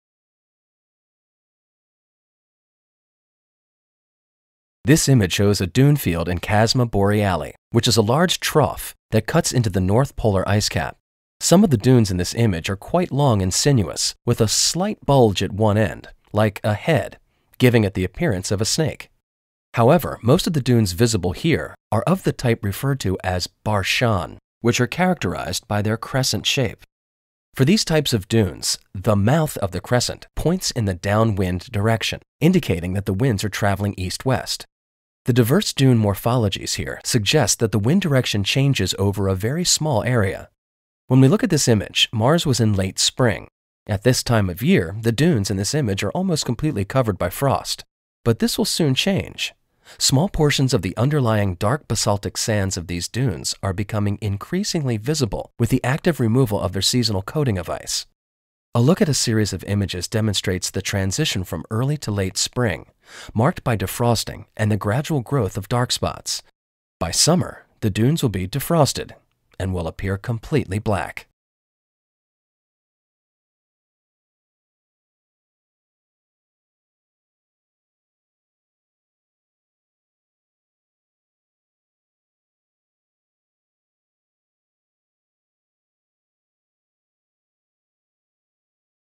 HiClip: Defrosting Dunes within Chasma Boreale
This image shows a dune field in Chasma Boreale, which is a large trough that cuts into the North Polar ice cap.

NASA/JPL/University of Arizona (Audio Black and white images are 5 km across; enhanced color images are 1 km.)